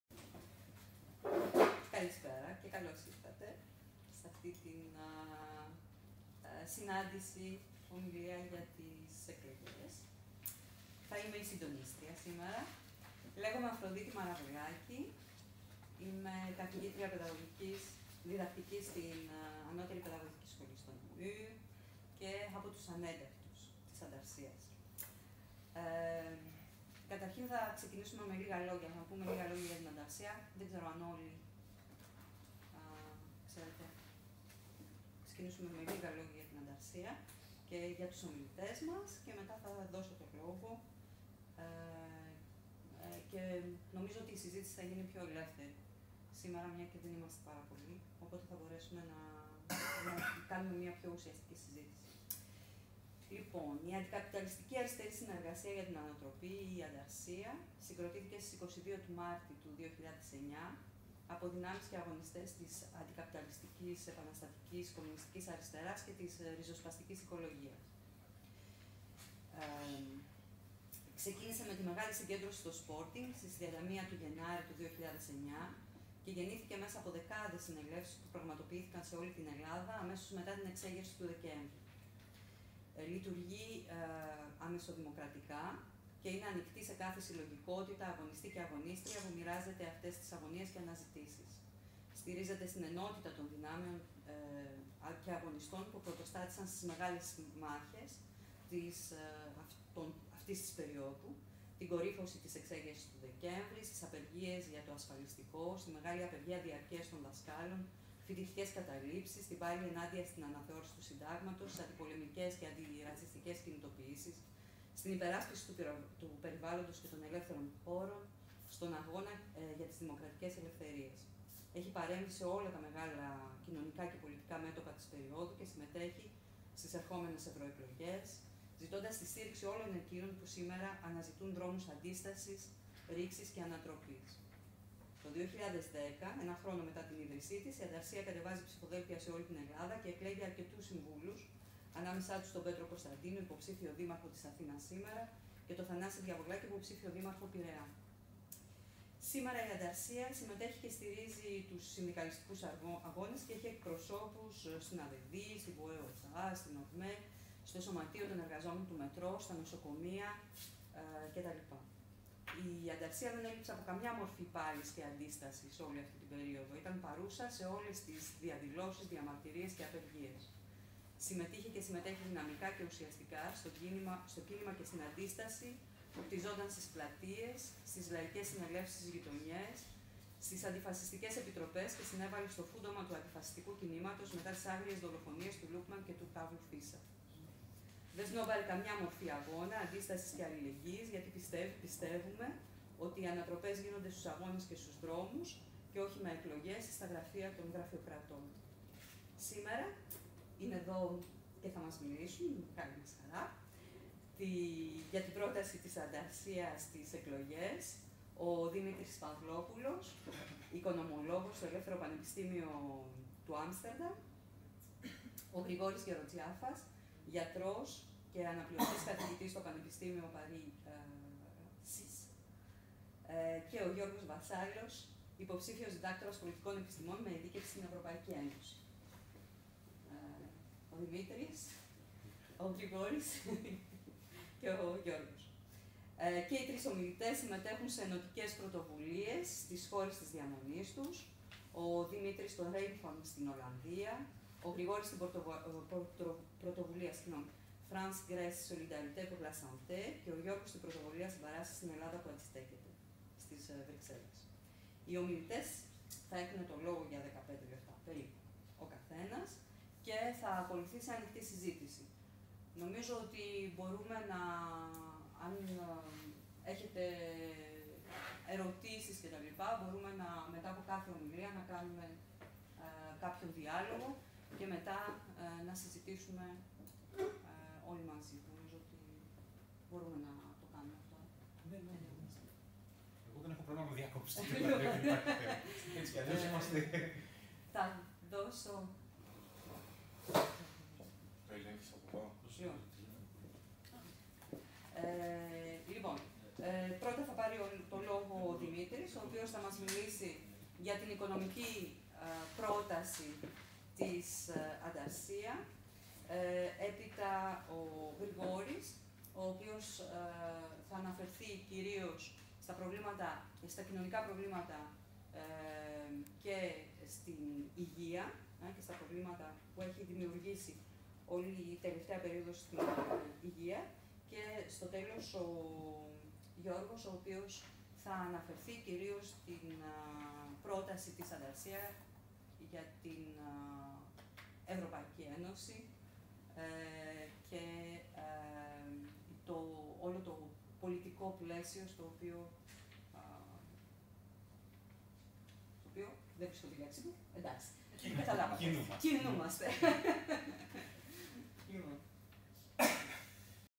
Εκδήλωση ΑΝΤΑΡΣΥΑ Βελγίου για εκλογές 1
Εισαγωγή από την σ. Αφροδίτη Μαραβελάκη, καθηγήτρια παιδαγωγικής στη Namur